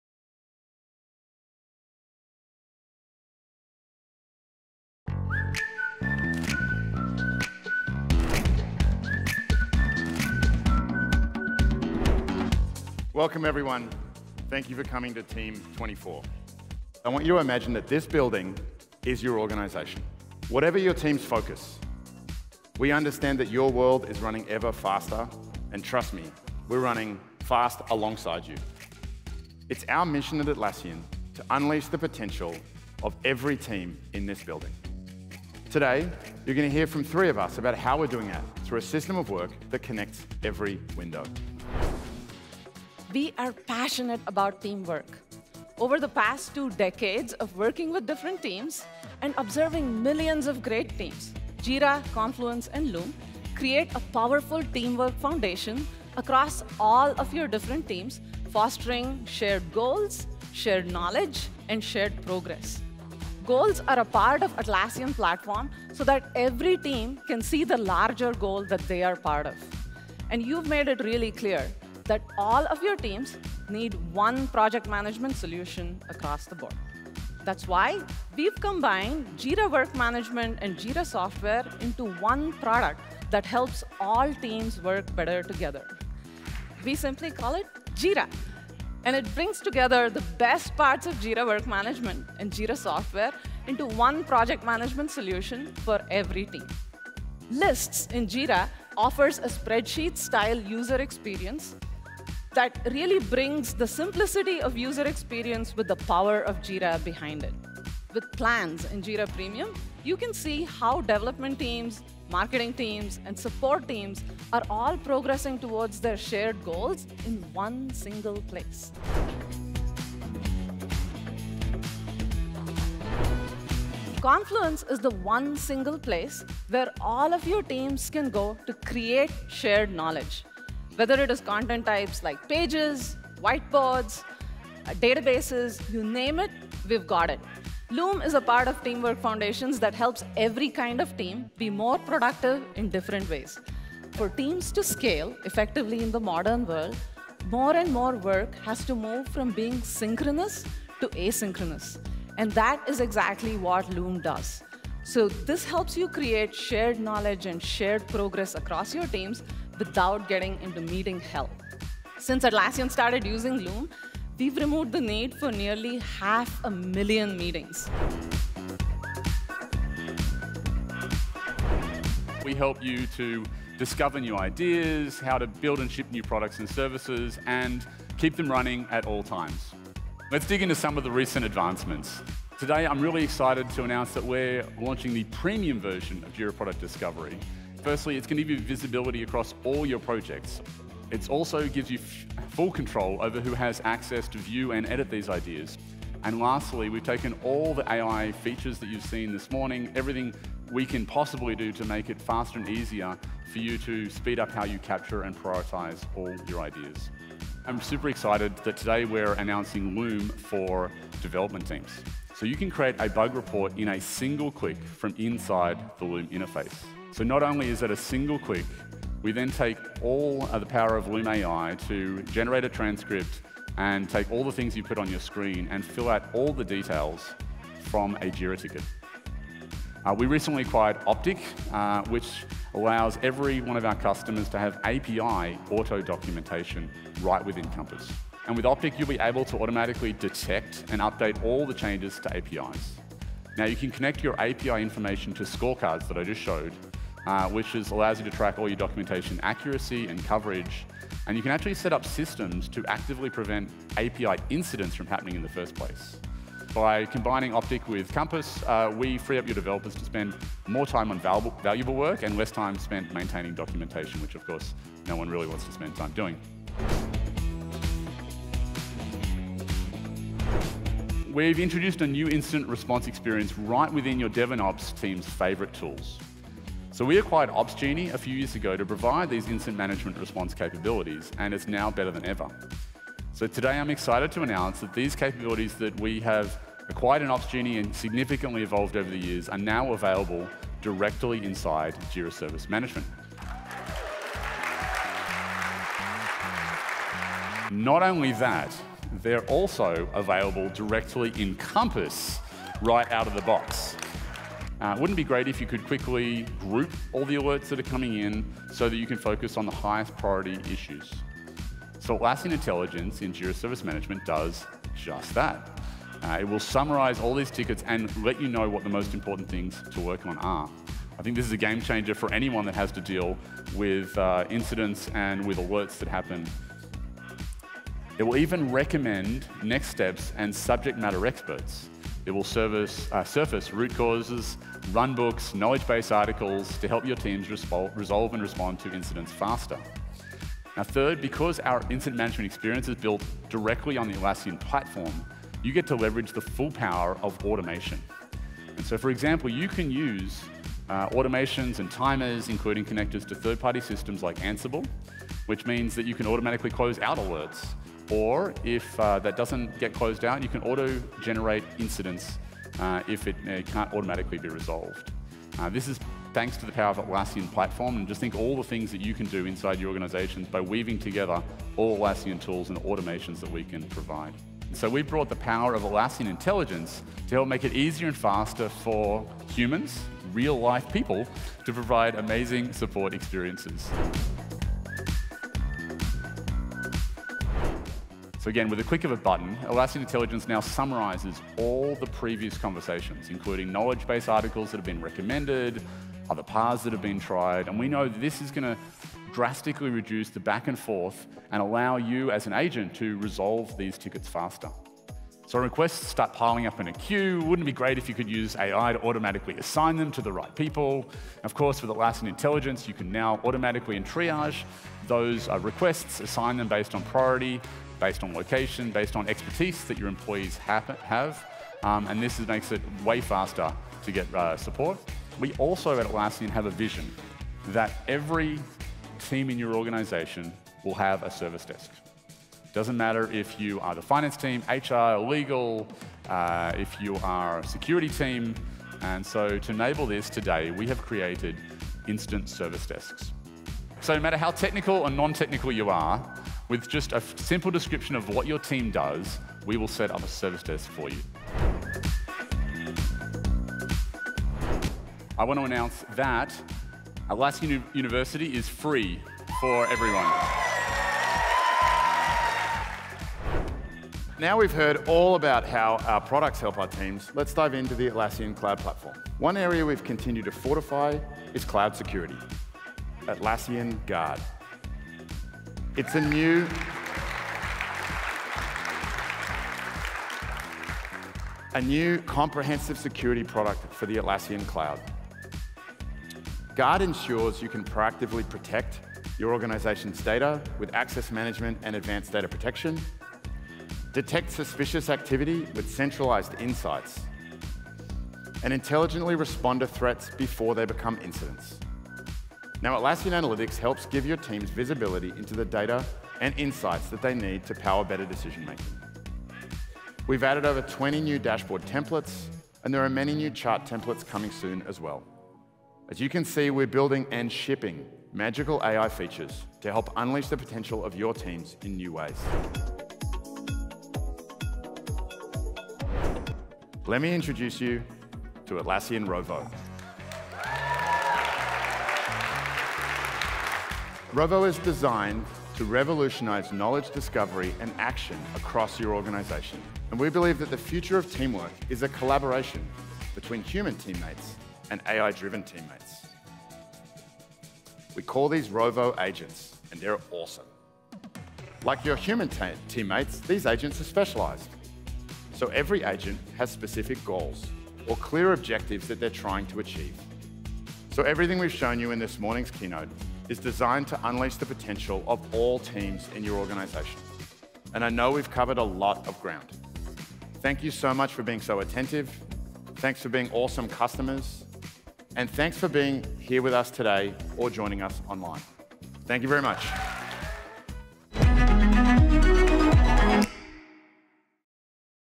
Team '24 – highlights | Atlassian
Distributed teams, artificial intelligence, and work silos are just a few of the challenges organizations face. We think technology should make work easier, not harder, and that connected teams equal successful teams.

Join founders Mike Cannon-Brookes and Scott Farquhar, and President Anu Bharadwaj, to hear how Atlassian will power the future of teams to make the impossible, possible.

Highlights:
0:00 Intro, Atlassian's system of work
0:41 Jira for all teams
2:00 New AI features in Confluence and Loom
2:50 Premium Jira Product Discovery
3:28 Loom for development teams
3:50 Optic acquisition
4:15 Optic + Compass
4:31 New incident response capabilities
5:11 Atlassian Intelligence
7:20 Instant service desks
7:35 Atlassian University is free for all
7:50 Atlassian Guard - cloud security
8:48 Atlassian Rovo, Rovo Agents

Speakers:
Mike Cannon-Brookes, Co-founder and co-CEO, Atlassian
Scott Farquhar, Co-founder and co-CEO, Atlassian
Anu Bharadwaj, President, Atlassian